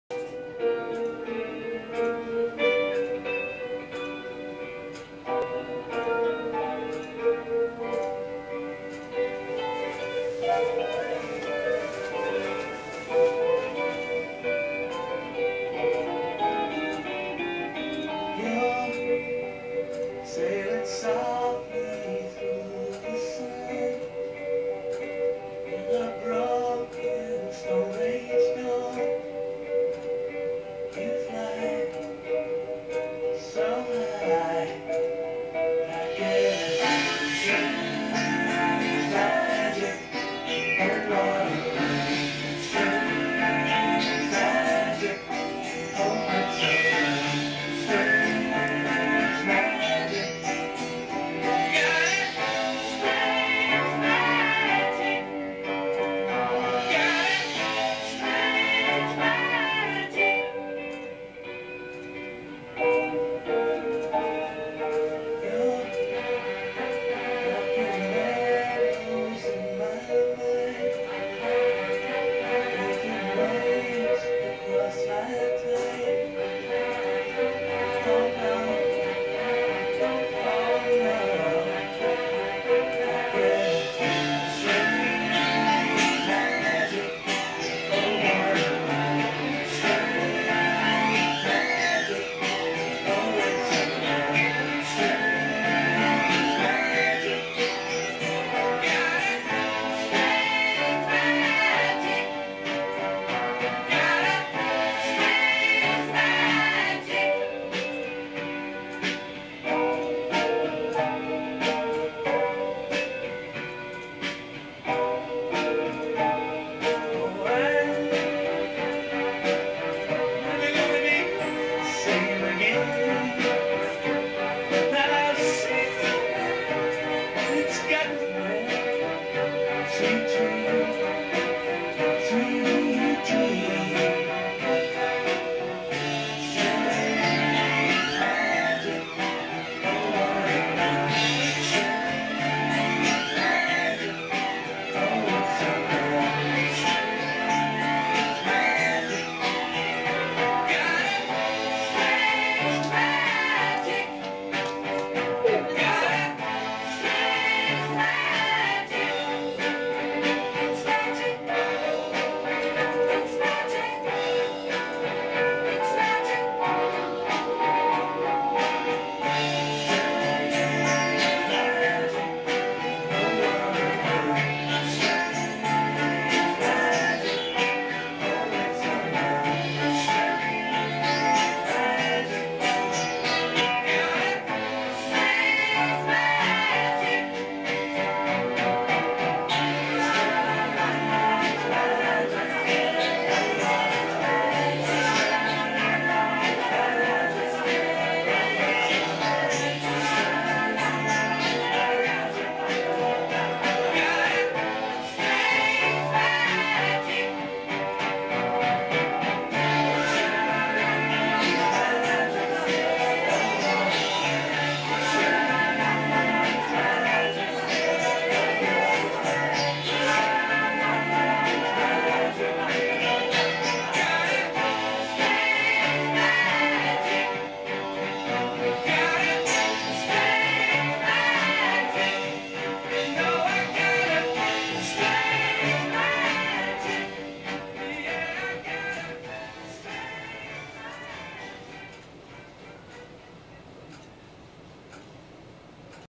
Baby Mantis Hatching
Mantis nymph emerging from an egg case.  Yes, praying mantises love Electric Light Orchestra.  If you'd like to see my pictorial write-up of setting up the hatching environment, go